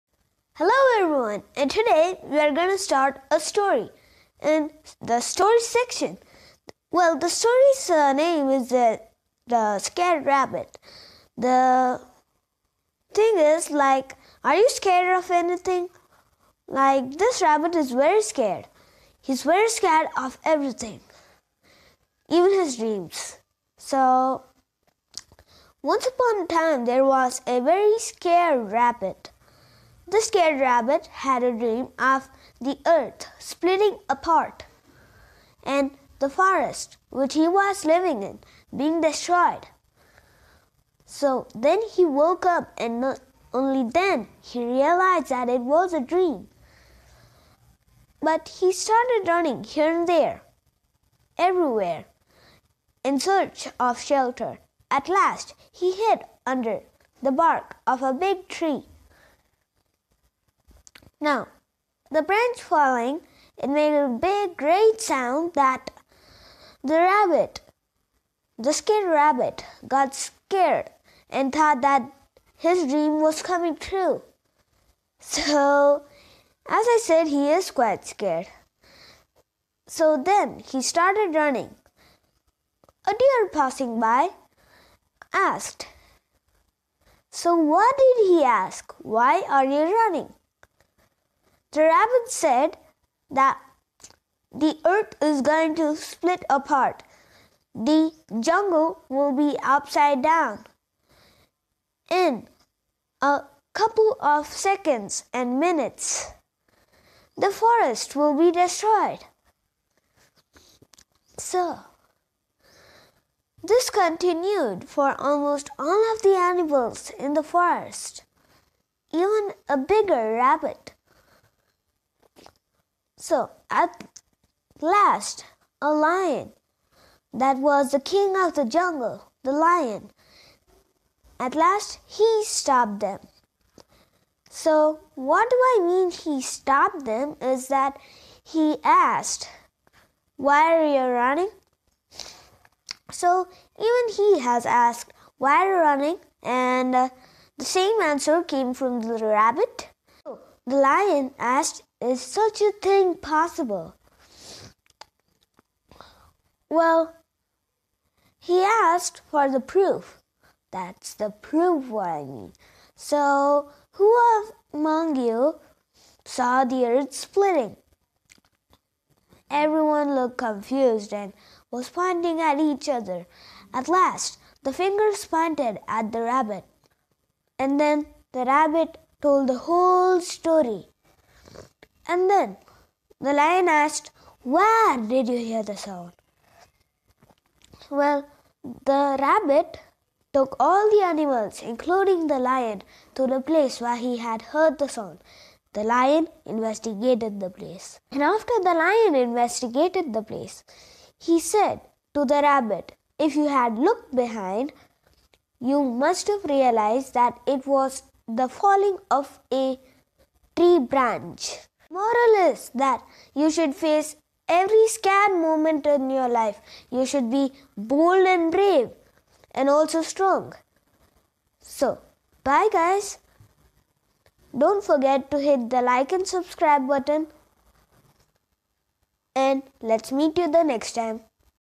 Interesting story "The Scared Rabbit". Be Brave and strong to face the problems
Story telling session. Interesting story "The Scared Rabbit".  Be Brave and strong to face the problems